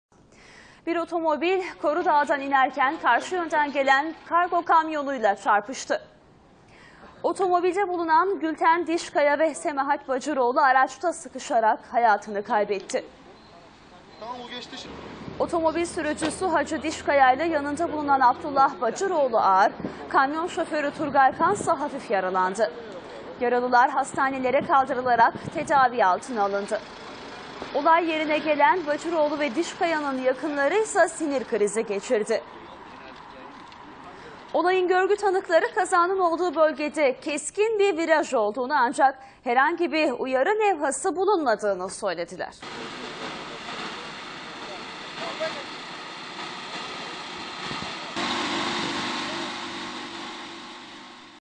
Çanakkale'de feci kaza!
Çanakkale'nin Gelibolu ilçesinde meydana gelen trafik kazasında kargo kamyonunun çarptığı otomobilde 2 kişi feci şekilde can verirken 2'si ağır 3 kişi yaralandı